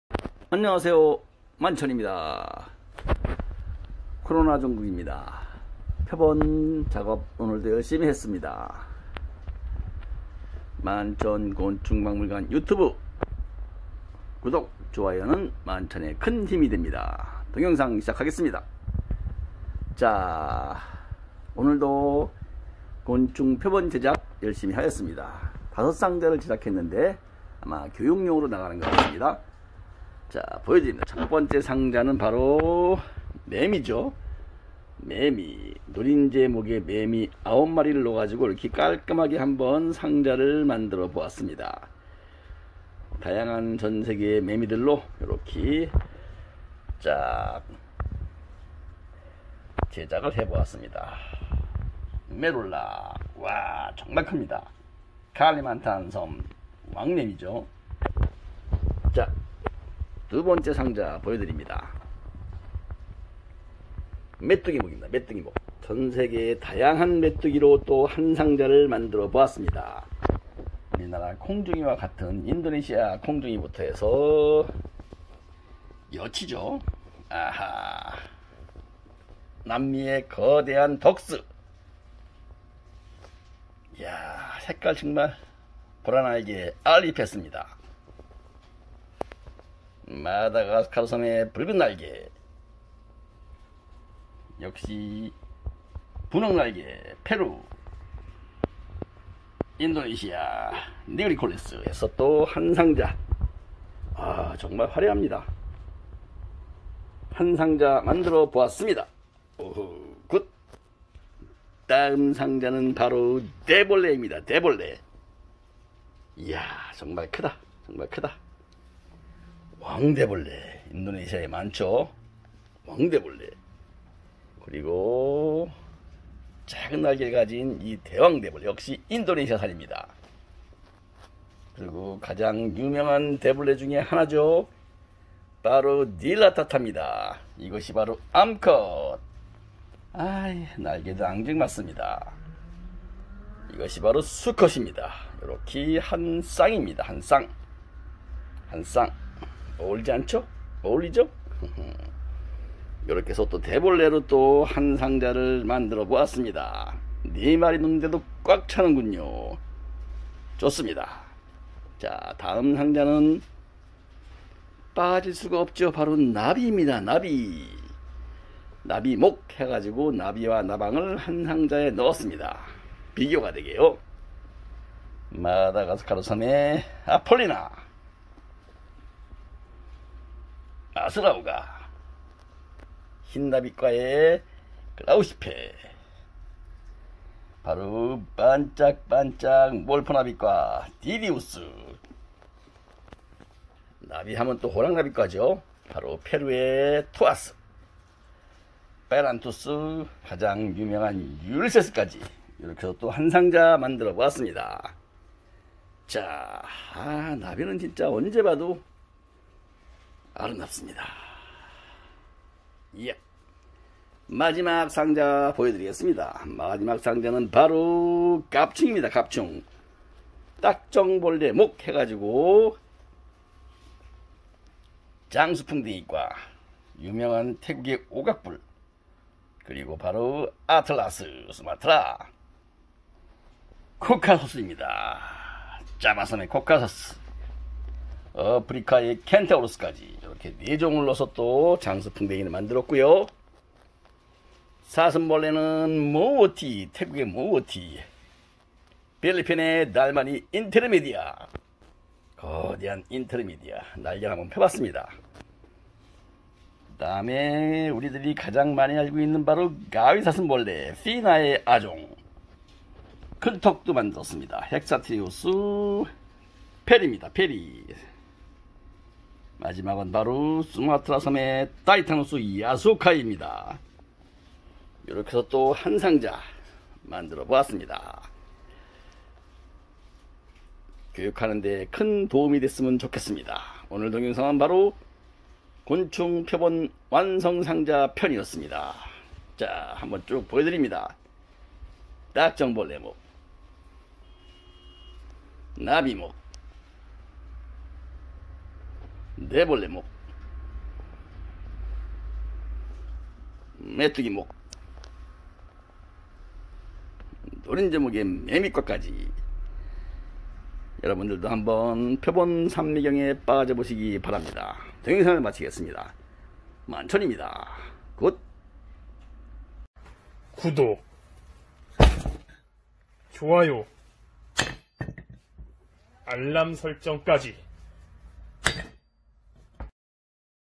곤충표본완성상자 다양하게 만들어보기!! 교육용 곤충표본완성상자 꾸며보기~ ♡
노린재목, 메뚜기목, 대벌레목, 나비목, 딱정벌레목
완성상자 만들기~~~♡